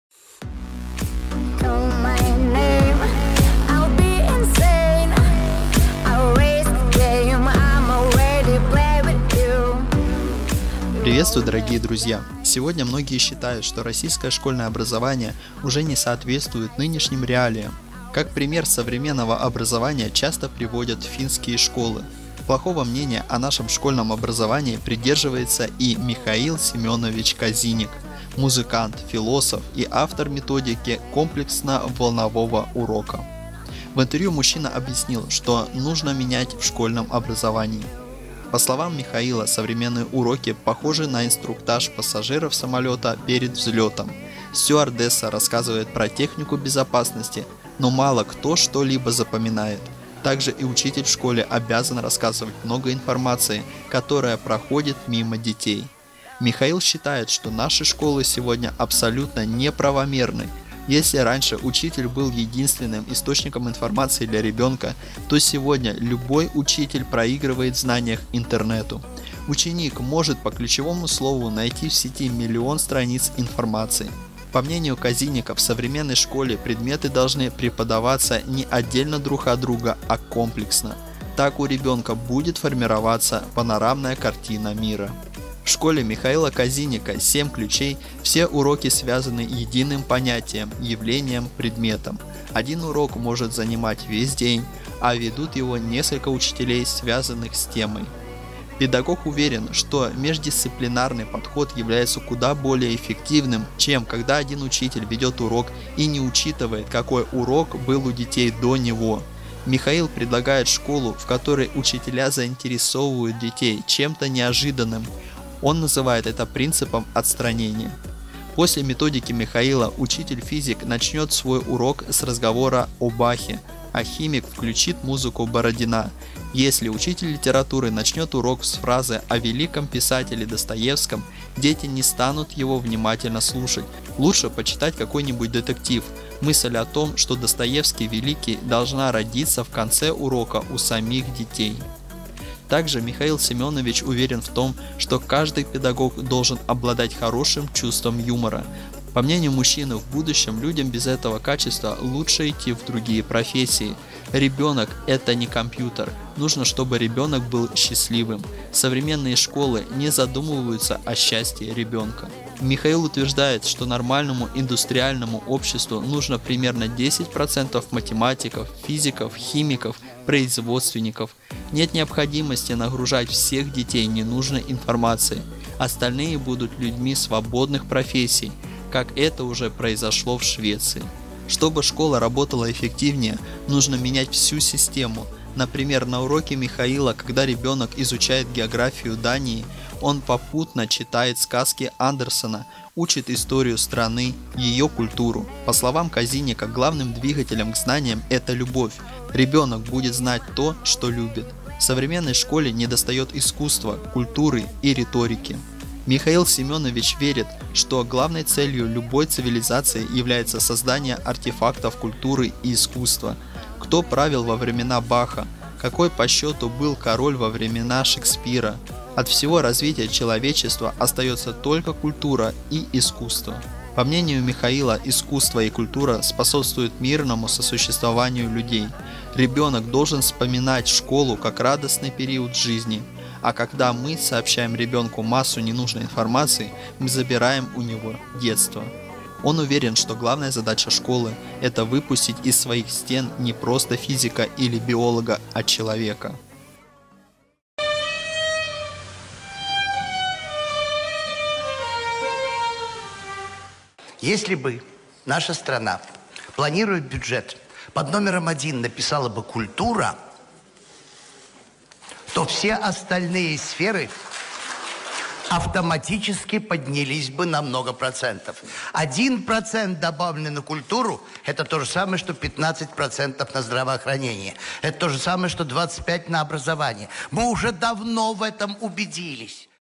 Михаил Казиник о современной школе
«Современная школа порождает дичайшую необразованность»